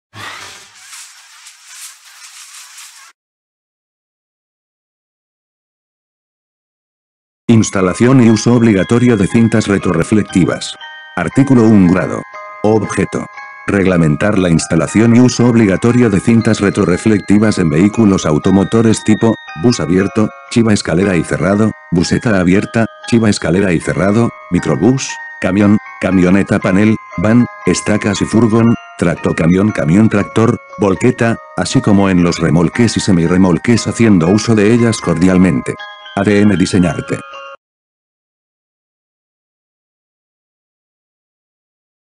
Resolución 0003246 cinta reflectiva
Instalación y uso obligatorio de cintas retrorreflectivas.
“Artículo 1°. Objeto. Reglamentar la instalación y uso obligatorio de. cintas retrorreflectivas en vehículos automotores tipo: bus abierto, chiva escalera y cerrado, buseta abierta, chiva escalera y cerrado, microbús, camión, camioneta panel, van, estacas y furgón, tractocamiòn Camión tractor, volqueta, así como en los remolques y semirremolques haciendo uso de ellas.
cordialmente. A D M Diseñarte